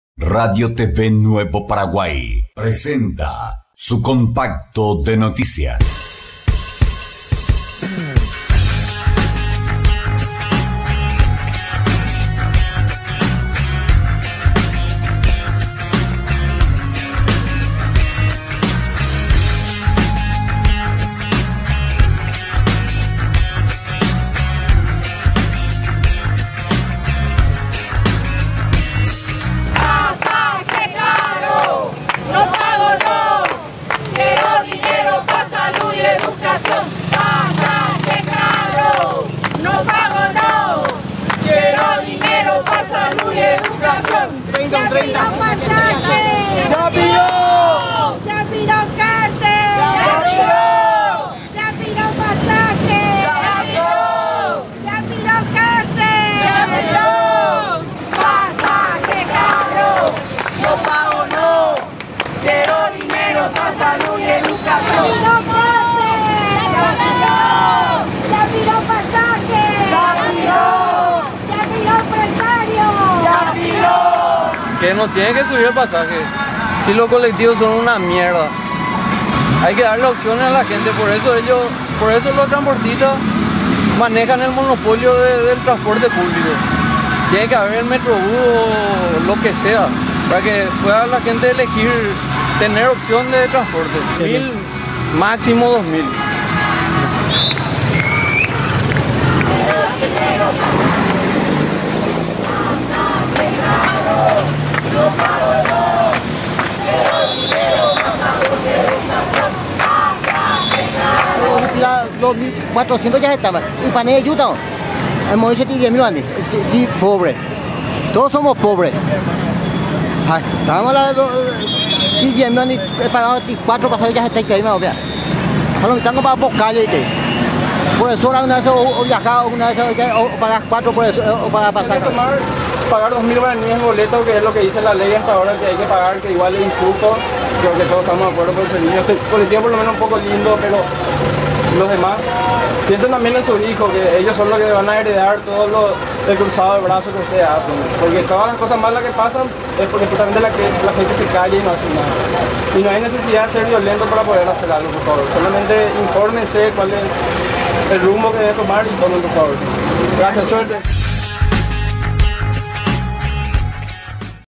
Nueva Jornada de Protesta
Nueva protesta en Autopista y Madame Lynch
La ciudadanía no para con sus protestas por la suba del pasaje a 2.400 Gs. Y por el pésimo servicio.